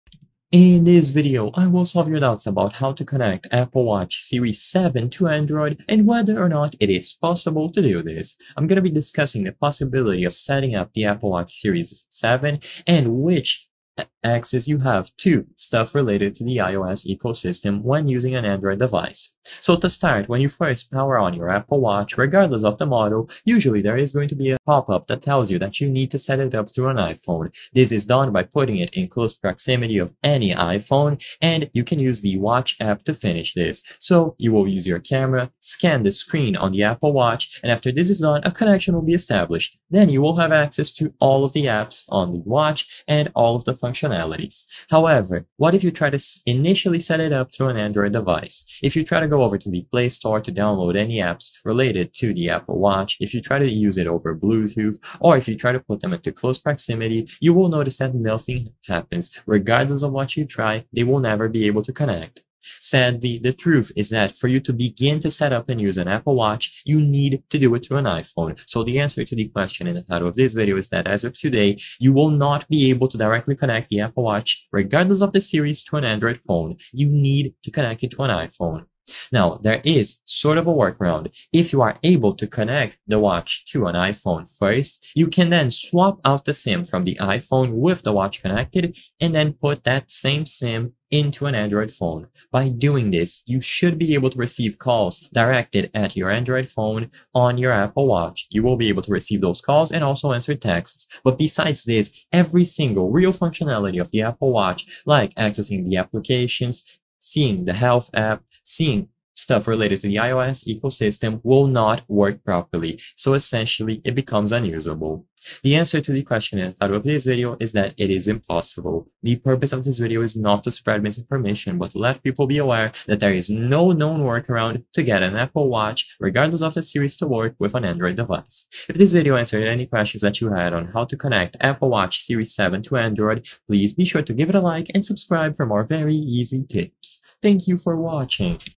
How To Connect Apple Watch Series 7 to Android? (2025)
I will solve your doubts about how to connect apple watch series 7 to android, and whether or not it is possible to do this.

I hope I have helped you with the video!